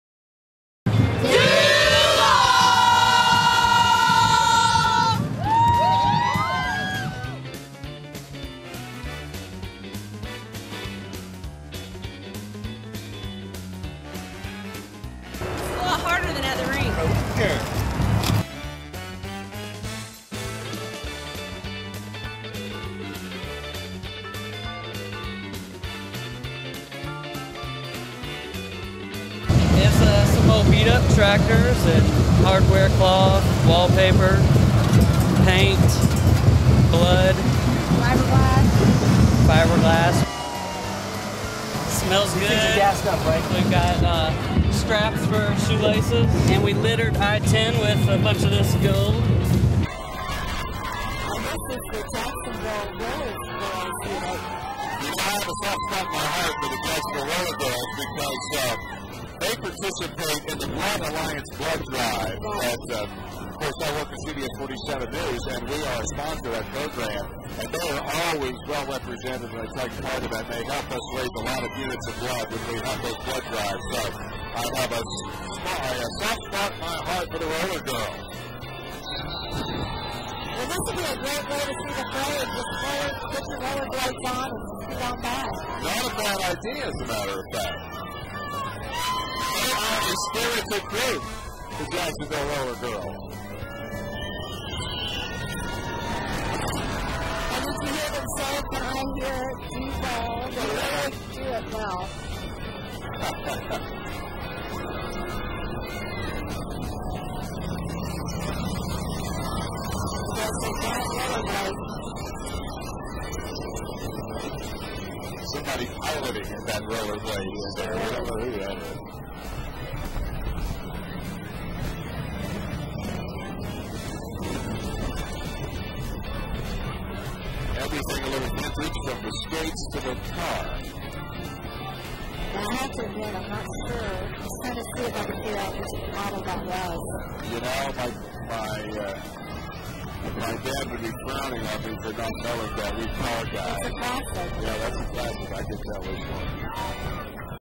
Jacksonville RollerGirls in Gator Bowl Parade
12/31/08 Winn-Dixie Hometown Gator Bowl Parade Jacksonville, FL (broadcast footage: Comcast) JacksonvilleRollerGirls.com